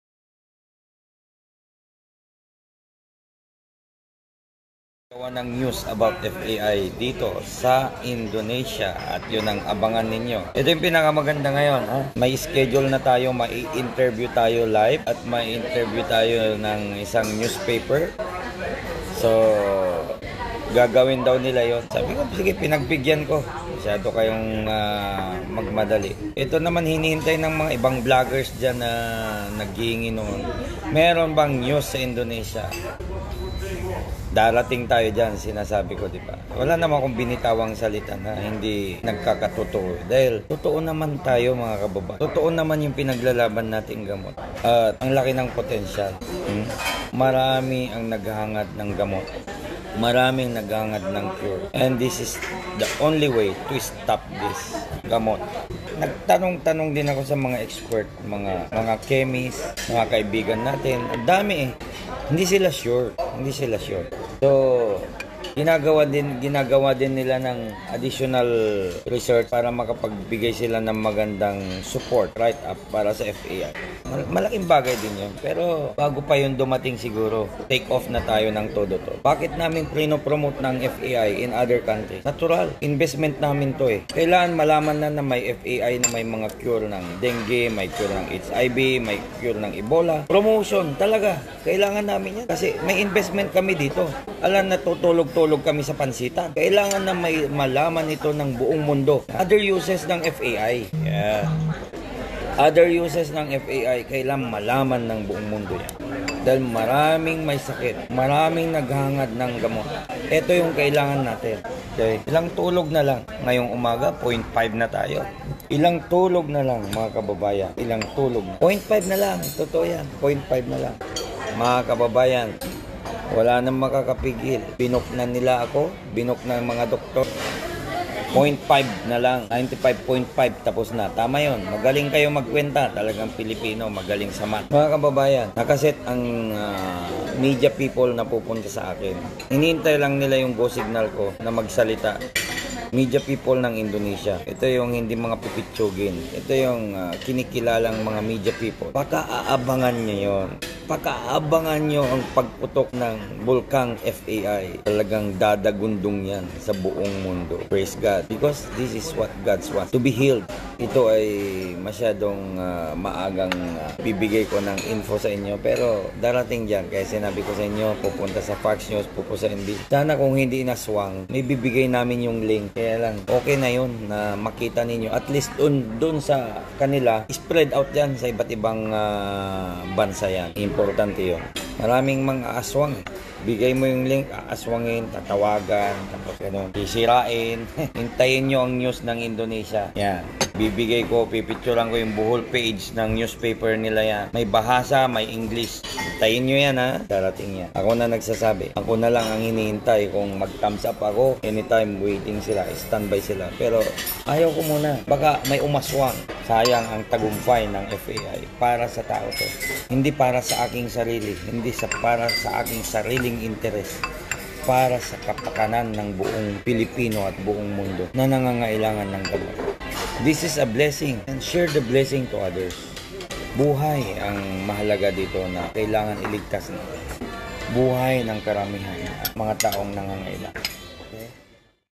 (FAI)RED CARPET KAY MARIO MARCOS ?-DANSUNT COLLECTION PHILIPPINES
ALAMIN KUNG BAKIT BIBIGYAN NG RED CARPET SA PALASYO
SI MR.MARIO MARCOS ?
HINDI NA MAPIGILAN ANG PAGLAGANAP NG BISA NG
FABUNAN ANTIVIRAL INJECTION SA INDONESIA.

SALAMAT PO NG MARAMI SA LAHAT
GOD BLESSED